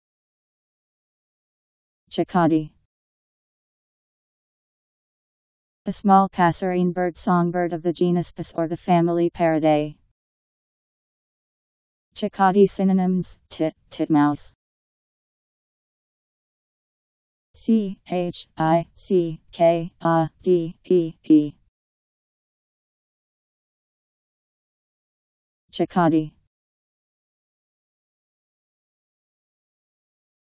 Chickadee Meaning
Video shows what chickadee means. A small passerine bird (songbird) of the genus Parus or the family Paridae.. chickadee synonyms: tit, titmouse.  chickadee pronunciation. How to pronounce, definition by Wiktionary dictionary. chickadee meaning. Powered by MaryTTS